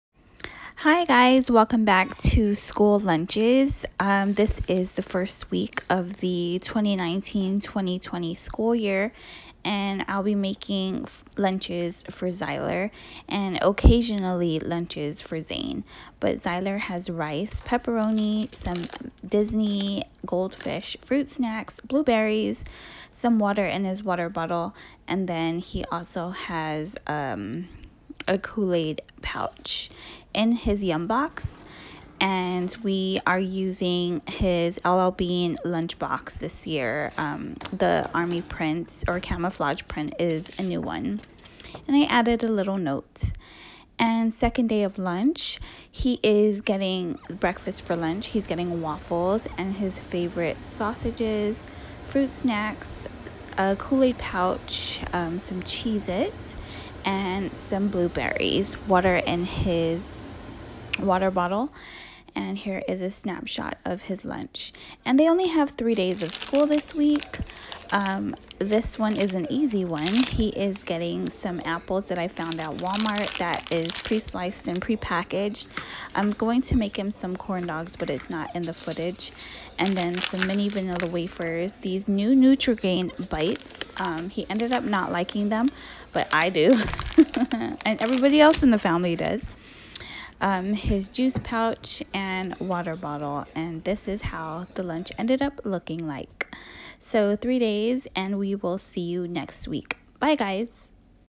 Kids School Lunches Week 1
Here is the first week of school lunches for school year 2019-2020.